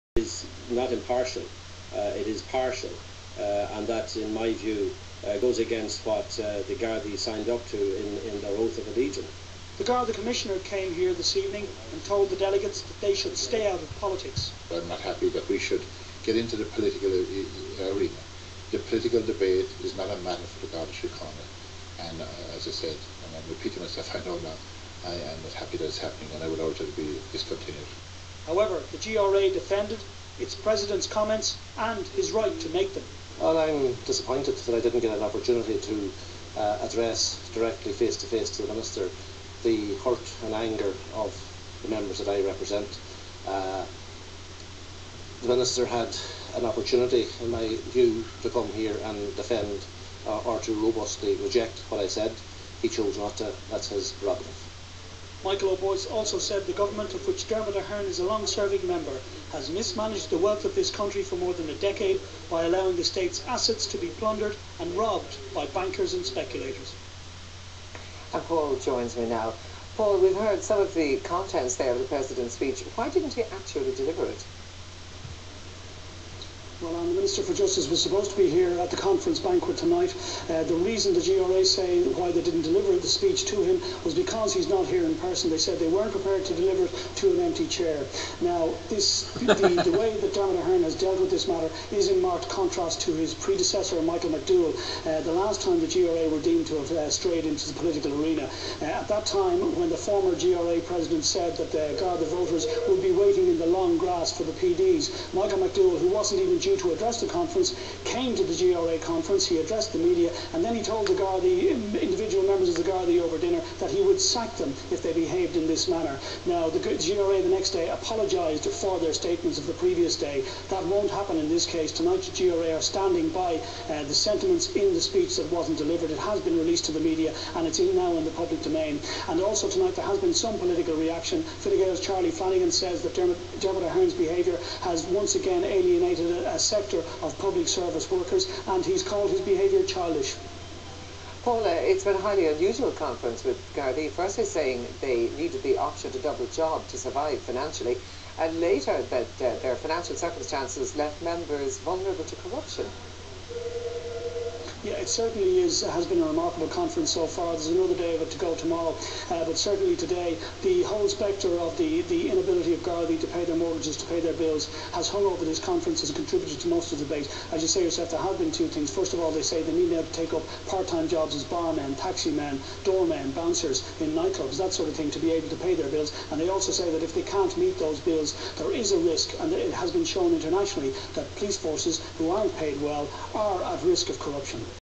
Ferryhouse77-dermot Ahern Responds to garda accusation.wmv
first we heard Ruiri Quinn accuse the Department of education in Cover up...now we hear the garda representative body accuse the Minister for Justice, Dermot ahern and his government of being "corruptors; and here we hear dermot ahern response;